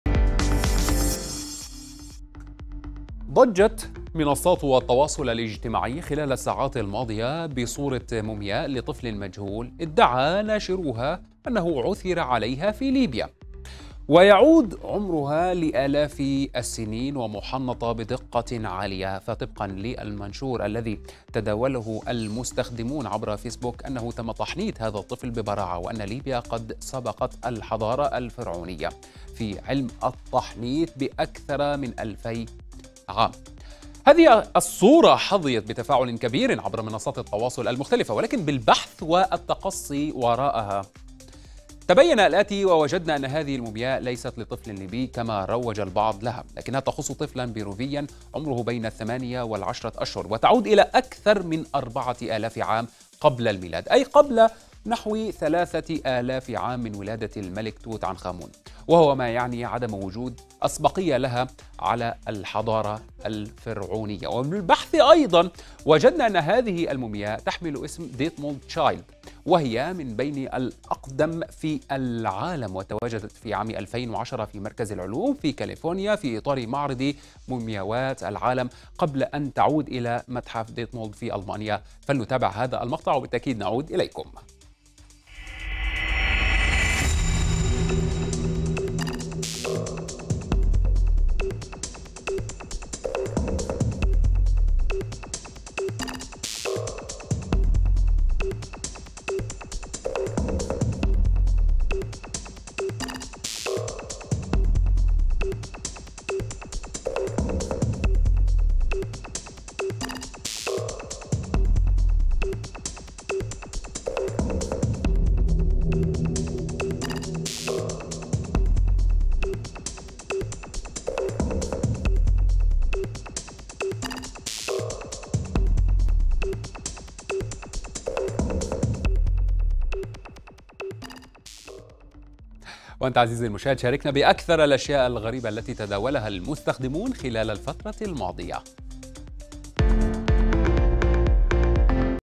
مومياء طفل عمرها آلاف السنين تثير الجدل في ليبيا.. فما حقيقتها؟
تنتشر على مواقع التواصل الاجتماعي صورة يدّعي ناشروها أنها تعود لمومياء طفل عمرها آلاف السنين عثر عليها في ليبيا.
وتبدو الصورة وكأنها مومياء لطفل قيل إنها من ليبيا، إلا أنّ هذه الصورة لا علاقة لها بليبيا وهي لمومياء بيروفيّة.
وكتب مشاركو الصورة في التعليق أنّها لمومياء "عُثر عليها في ليبيا وهي تعود لطفل لا يزيد عمره عن عامين". وأضافوا: "تم تحنيط هذا الطفل ببراعة… ومع هذا الاكتشاف تكون ليبيا قد سبقت الحضارة الفرعونية في علم التحنيط بأكثر من 2000 عام".

وكالعادة، حظيت الصورة بآلاف التفاعلات على صفحات مواقع التواصل الاجتماعي ولا سيّما الليبيّة منها.